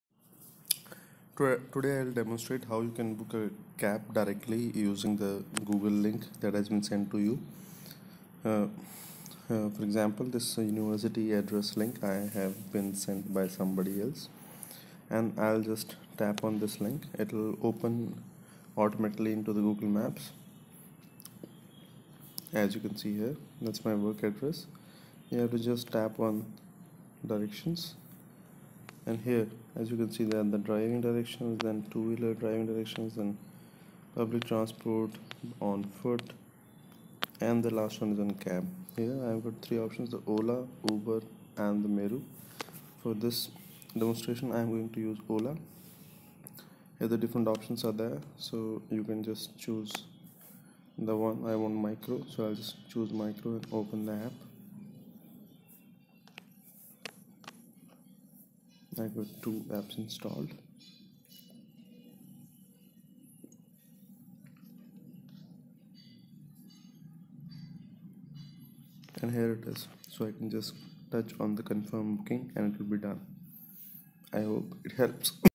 How to book cab using location sent to you using google map
This video helps you book a cab ride using google maps, when location has been sent to you.
Same procedure can also be used when your friend has senr you his/her location and you have to book a cab to go to him/her.